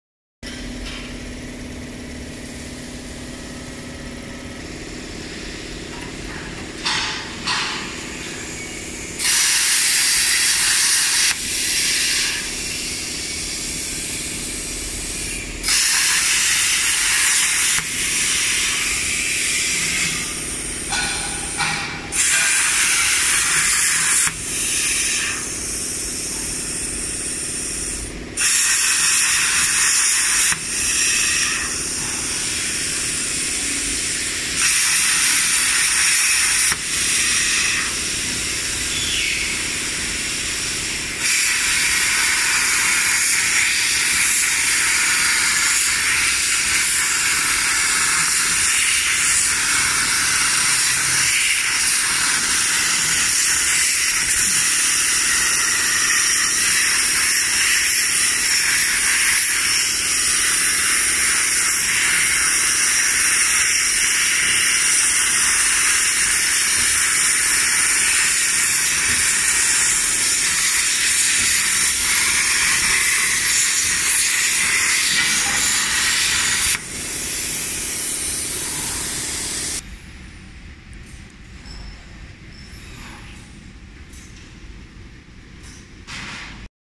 Affordable Gantry Type CNC Plasma Table from Jinan EagleTec
this is our gantry type cnc plasma cutting machine. The video is 2mm galvanized sheet cutting